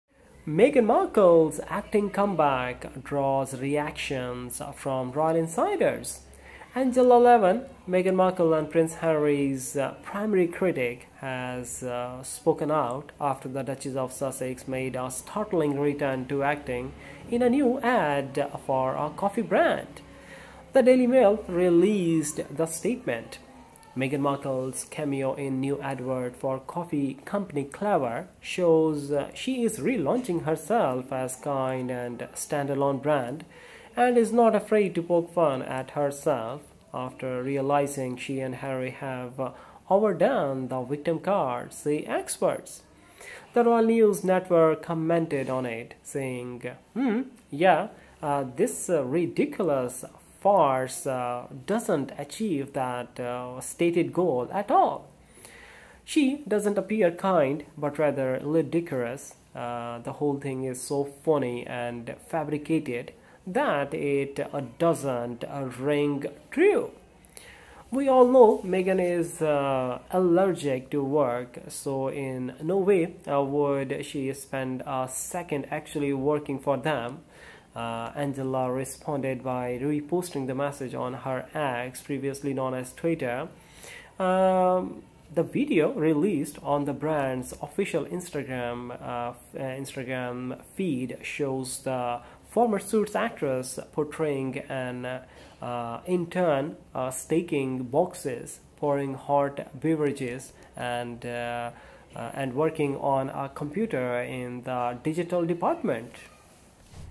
Meghan Markle's acting comeback draws reactions from royal insiders.
Angela Levin, Meghan Markle and Prince Harry's primary critic, has spoken out after the Duchess of Sussex made a startling return to acting in a new ad for a coffee brand.
The Daily Mail released the following statement: "Meghan Markle's cameo in new advert for coffee company Clevr shows she is relaunching herself as 'kind' and 'standalone' brand and is 'not afraid to poke fun at herself' after realising she and Harry 'have overdone the victim card', say experts."
The Royal News Network commented on it, saying, "Umm, yeah, this ridiculous farce doesn't achieve that stated goal at all." She doesn't appear "kind," but rather ludicrous. The whole thing is so phony and fabricated that it doesn't ring true.
"We all know Meghan is allergic to work so in no way would she spend a second actually working for them."
Angela responded by reposting the message on her X, previously Twitter page.
The video released on the brand's official Instagram feed shows the former Suits actress portraying an intern stacking boxes, pouring hot beverages, and working on a computer in the digital department.